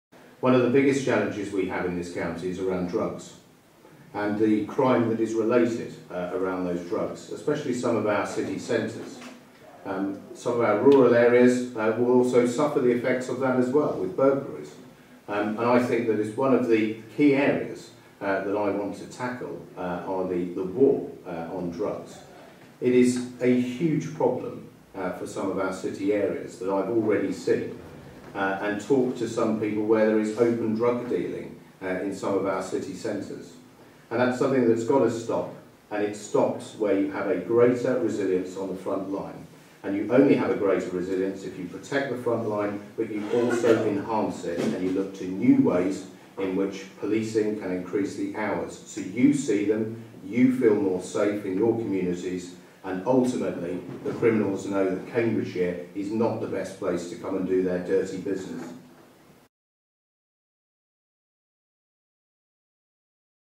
Conservative Police and Crime Commissioner Candidate Jason Ablewhite on Drugs
Cambridgeshire Police and Crime Commissioner Hustings, Great Shelford, 20 April 2016